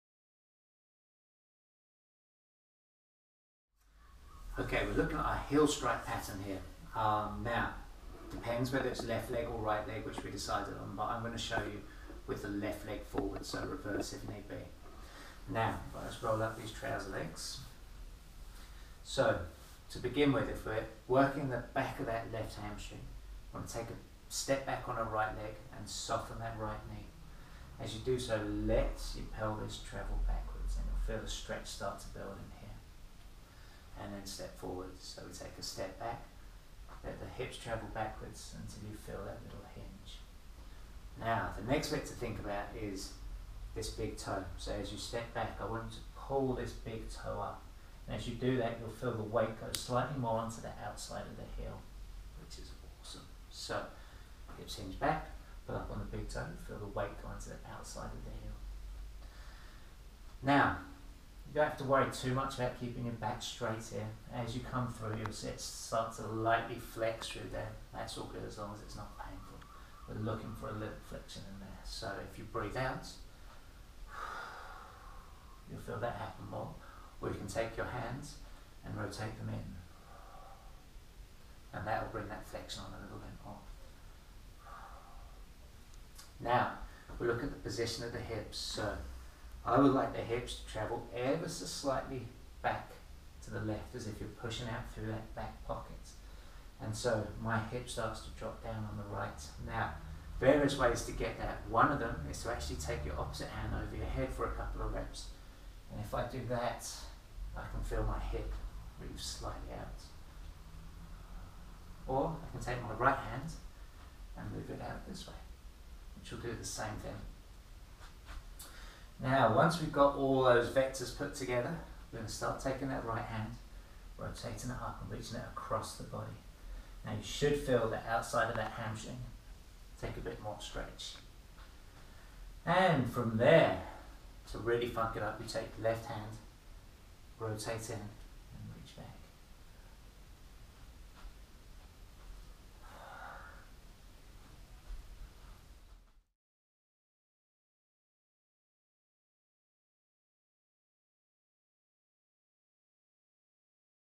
Anatomy in motion - Left heel strike
Having broken down the anatomy in motion gait phases into all the planes it's time to build them all back up. Here we look at left heel strike (often overlooked when rehabbing ankle sprains).